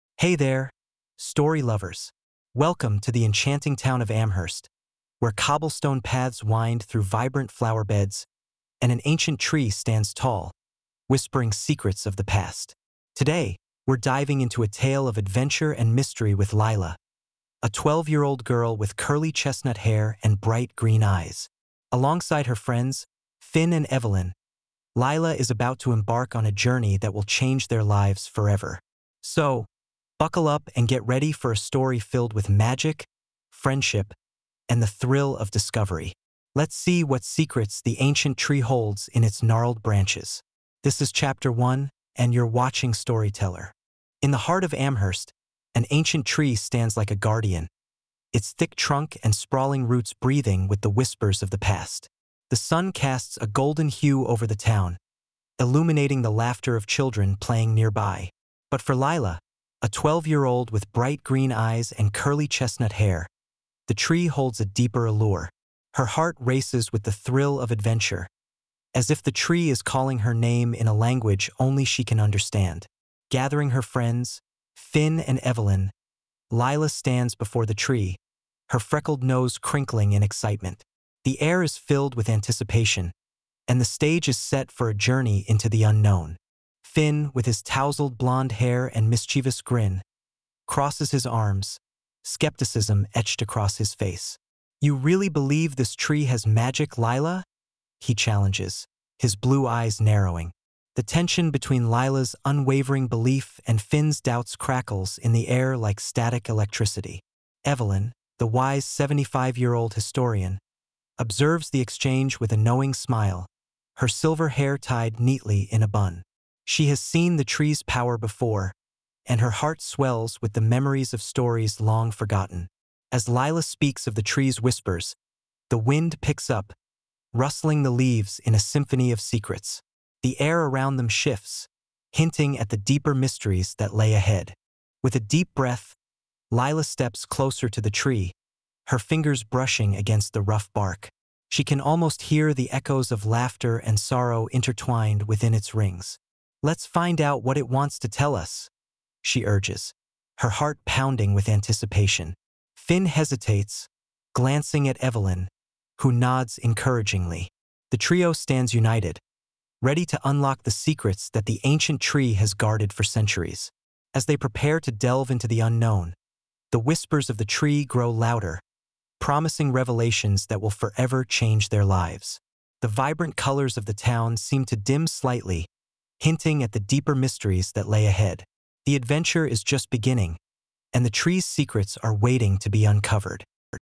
Secrets of the Ancient Tree: Discovering Magic | Ch 1
Join Lila and her friends as they stand before the ancient tree, ready to uncover its magical secrets. A journey filled with adventure, friendship, and discovery lies ahead!

In this enchanting chapter, we meet Lila, a twelve-year-old girl who feels an inexplicable pull towards the ancient tree in Amherst. As she embarks on an adventure filled with excitement and mystery, Lila gathers her friends Finn and Evelyn to explore what lies beneath the gnarled branches. Together, they are about to uncover secrets that will change their lives forever.

Key Points:
- The magic of the ancient tree
- Lila's curiosity and determination
- Friendship and teamwork in discovery
- The thrill of uncovering hidden tales

Timestamps:
00:00 - Intro & Hook
00:45 - Meeting the Ancient Tree
01:30 - Gathering Friends
02:15 - The Call to Adventure